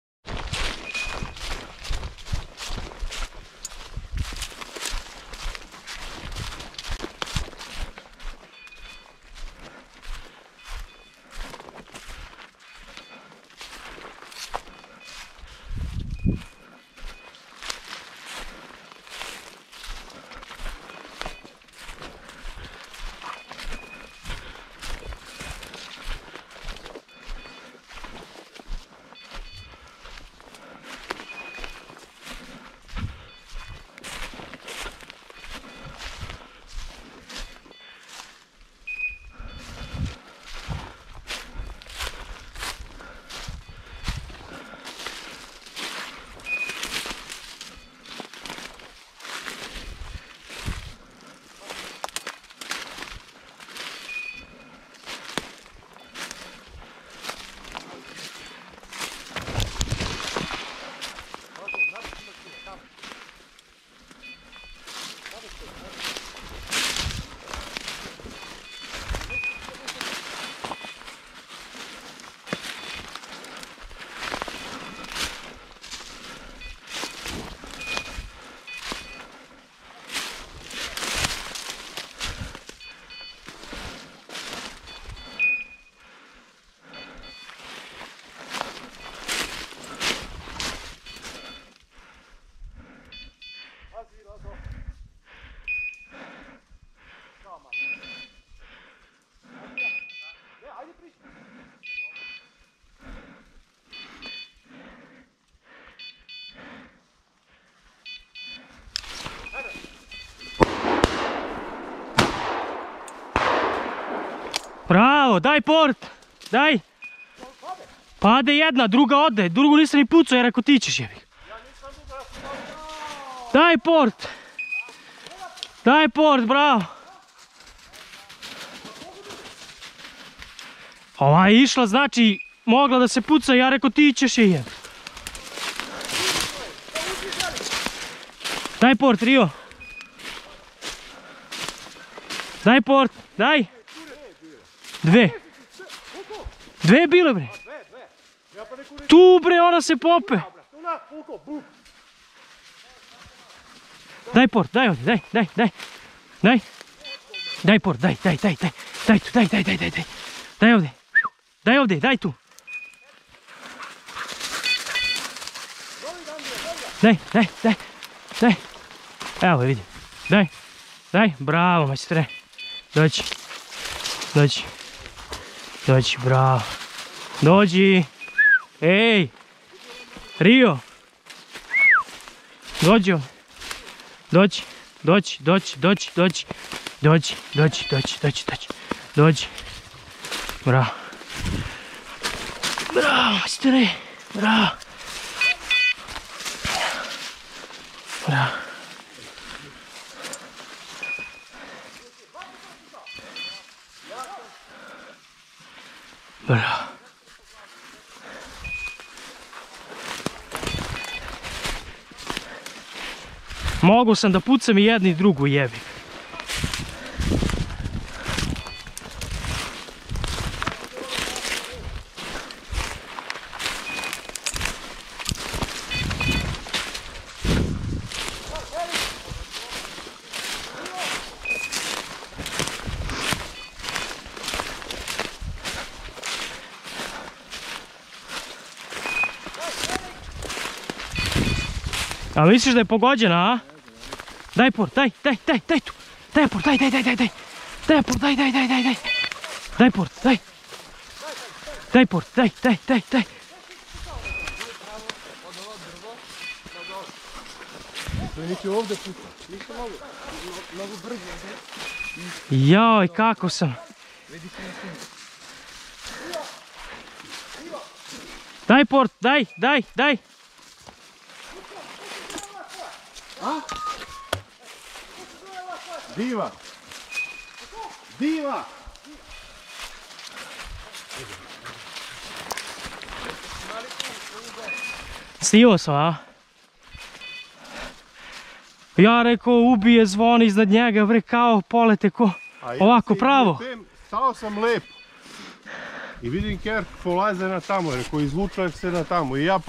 Epagneul breton Rio | Lov sljuke Leskovac Serbia
Beccaccia caccia epagneul breton
hunting woodcock